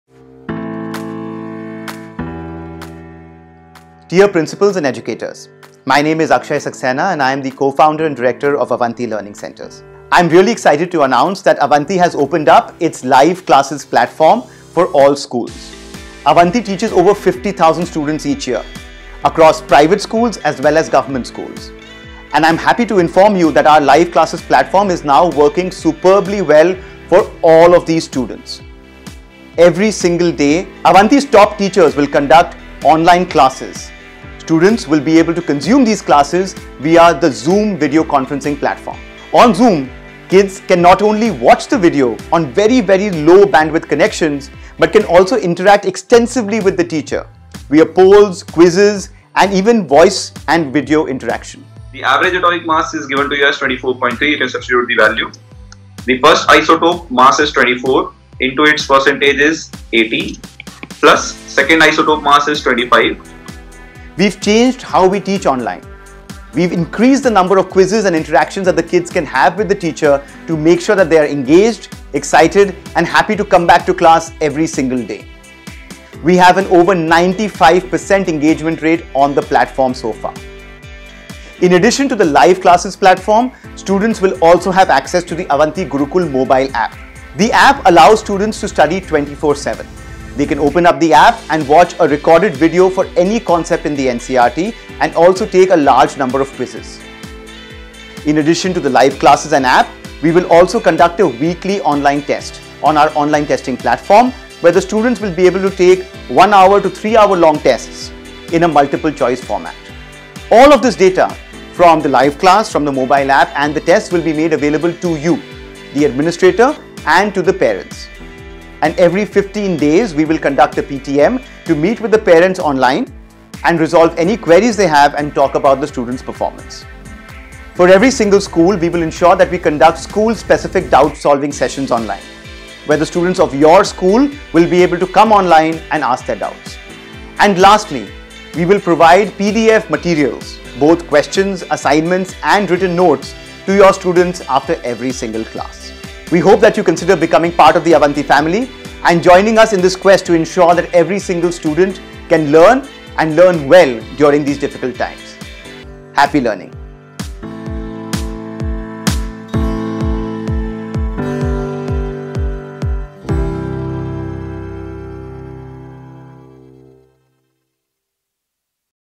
Avanti Live Classes for Schools: Enable exceptional digital at-home learning for your students
Avanti is opening up its Live Class platform for all Schools. For a small monthly fee your students will receive
- Access to daily Live Streaming classes by India's top educators
- Access to the Avanti Gurukul app for 24x7 learning through recorded topic-by-topic video content
- Weekly Online testing
- PDF notes and materials after each class

In addition the school and parents will receive fortnightly reports and will be invited to attend a PTM every two weeks to discuss their student's progress.